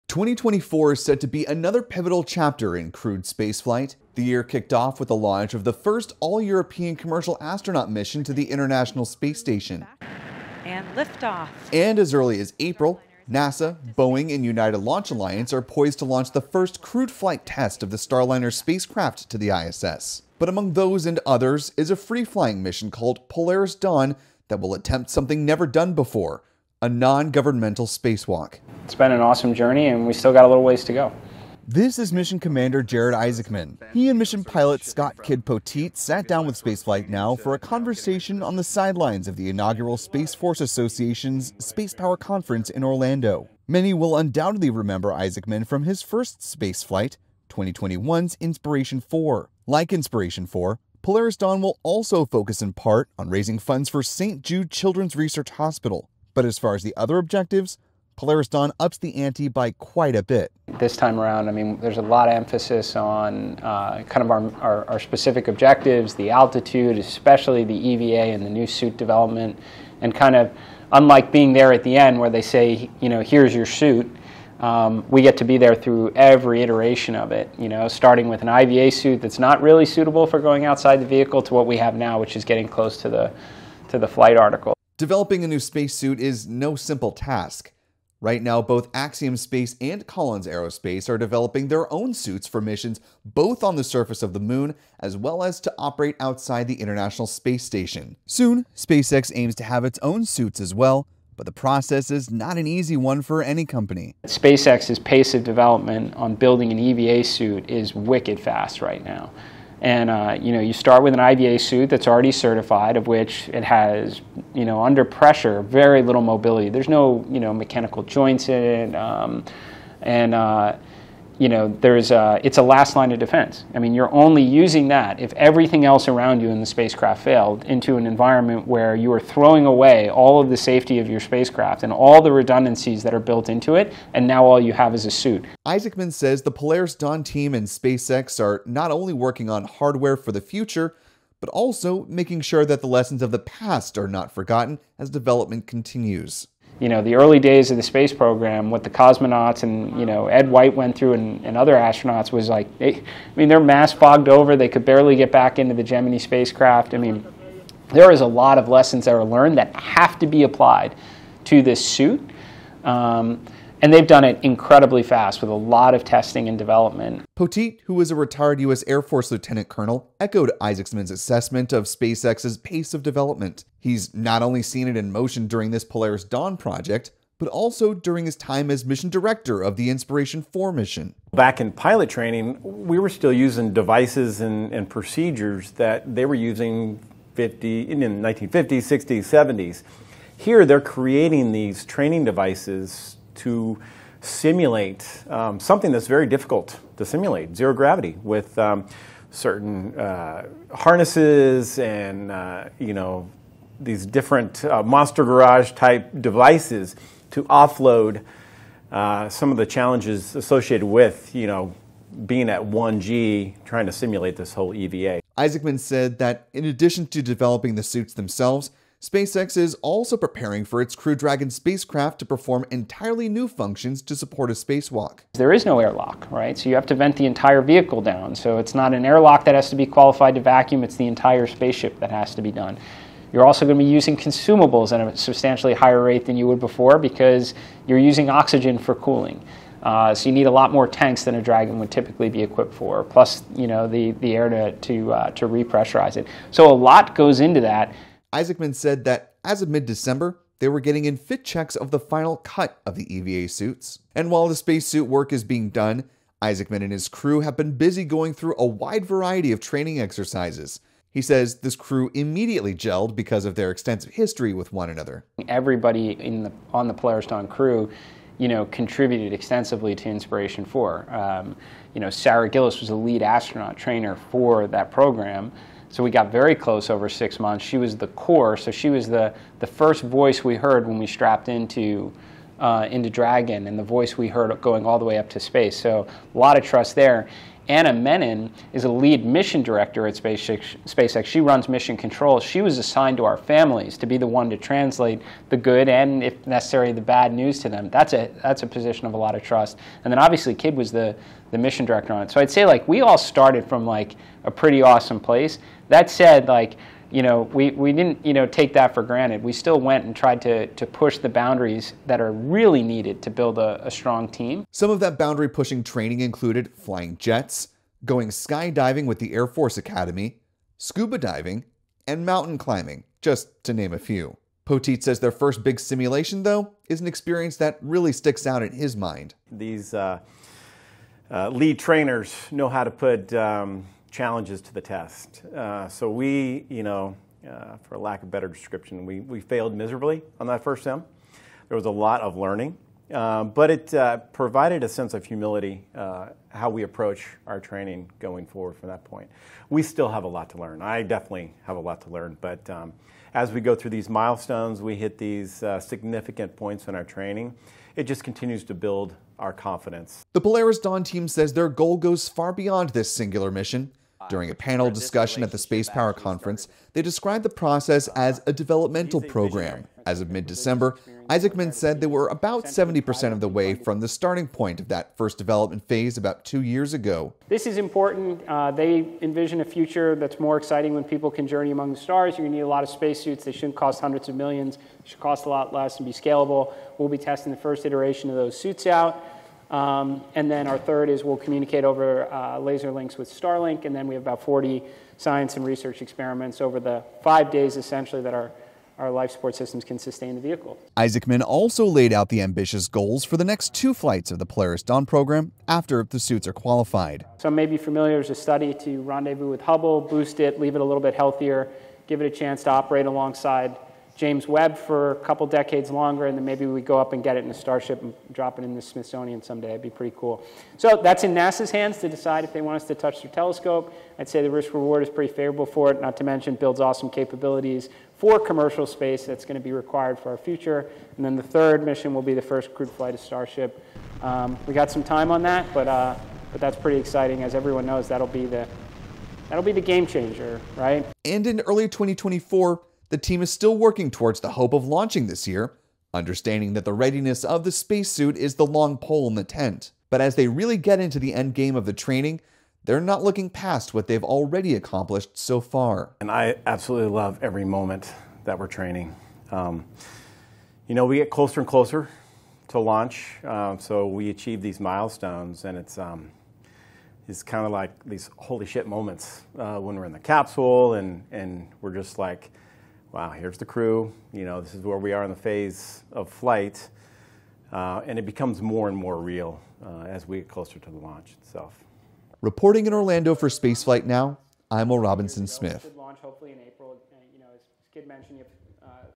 Polaris Dawn Commander, Pilot provide update on free-flying Dragon mission and SpaceX spacesuits
As SpaceX continues to fly both six-month crew rotation missions for NASA and private astronaut missions for Axiom Space, it's also working towards its next free-flying mission: Polaris Dawn.

This is the first of three planned, commercial astronaut missions for the Polaris Program, an initiative spearheaded by entrepreneur and Inspiration4 Commander, Jared Isaacman. He along with Pilot Scott "Kidd" Poteet, Mission Specialist Sarah Gillis and Mission Specialist/Medical Officer Anna Menon will conduct the first non-governmental spacewalk, exiting from a Crew Dragon spacecraft during a roughly 5-day mission.

Isaacman and Poteet recently spoke with Spaceflight Now's Will Robinson-Smith about the progress towards their mission and the state of the spacesuit development.

This coverage is made possible by the support of our members.